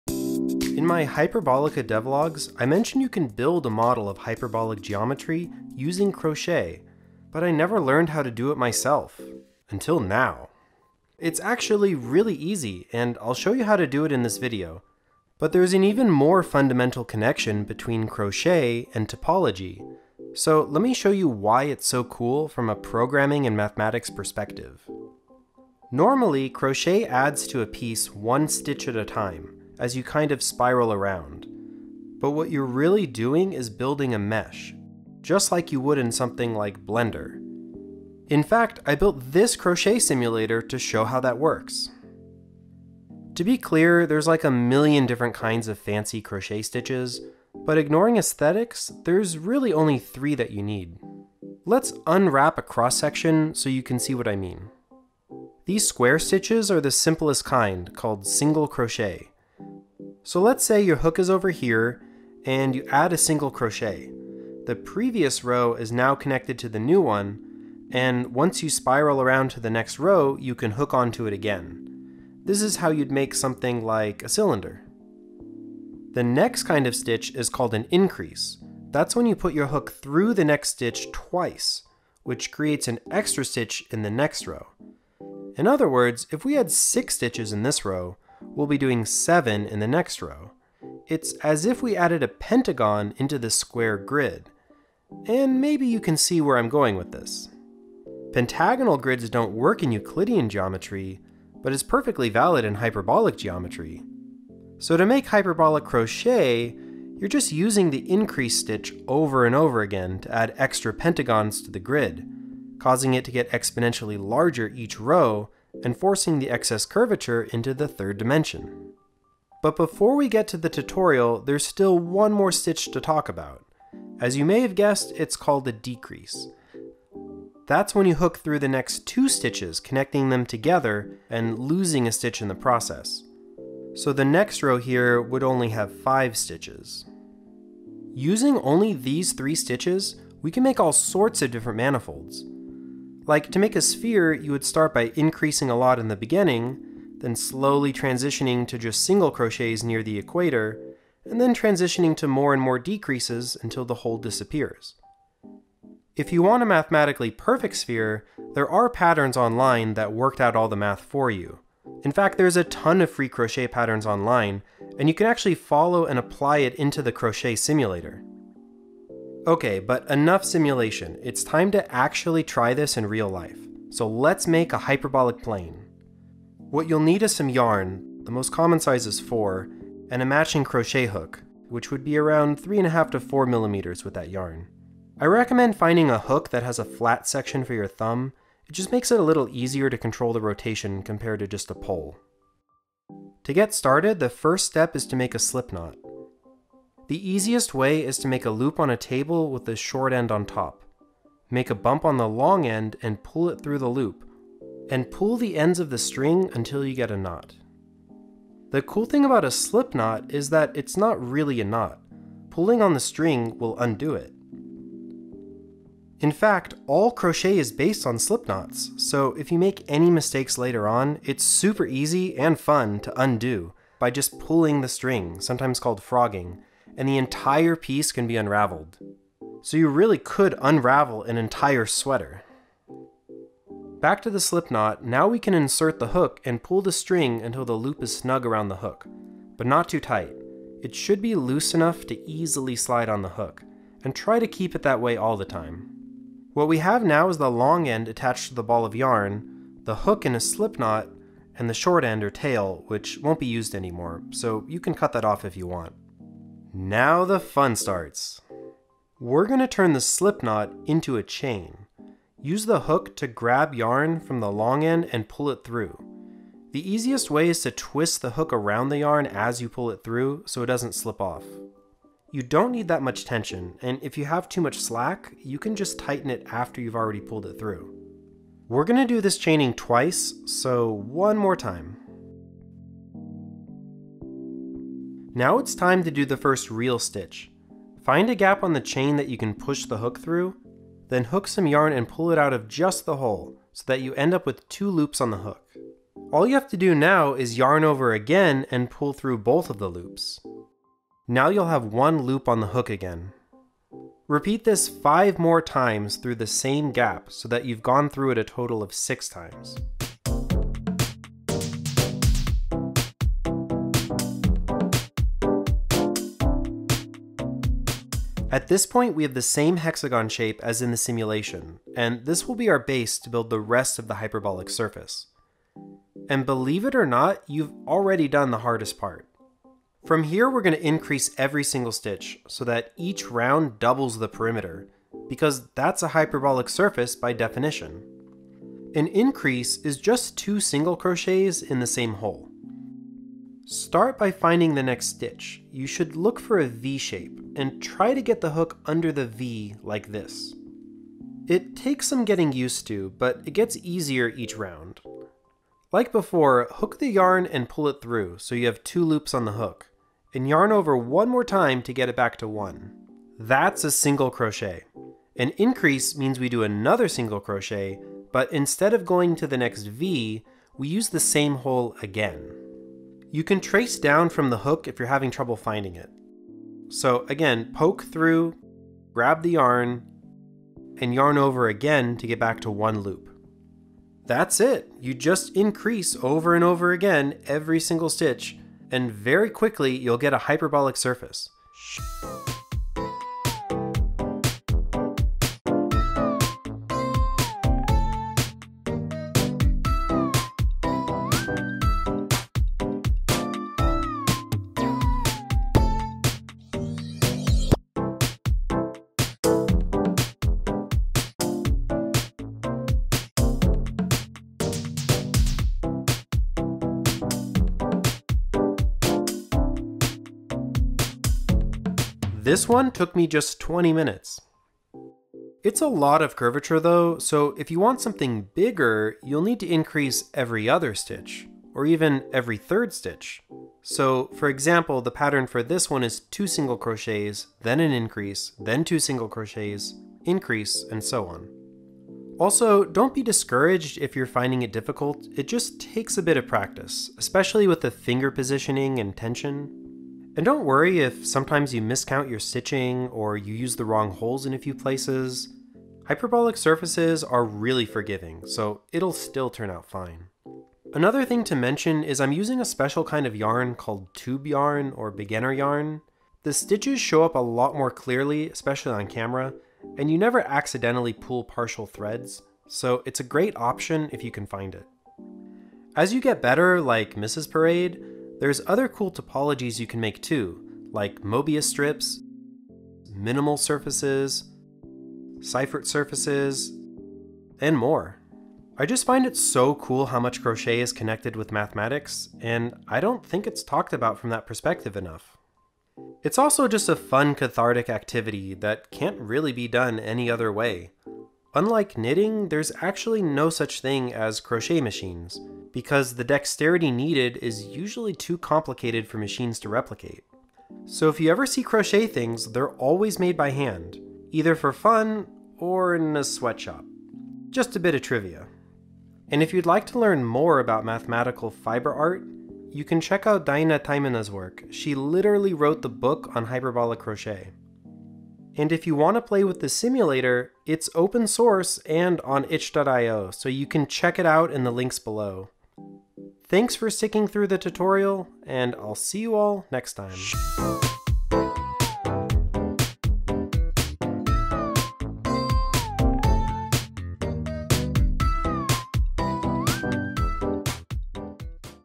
Make Your Own Hyperbolic Surface!
It's actually really easy and fun to make your own hyperbolic planes using crochet.  I had many requests for these after the release of Hyperbolica, so here's a proper tutorial for them.

Support me and innovative projects like these!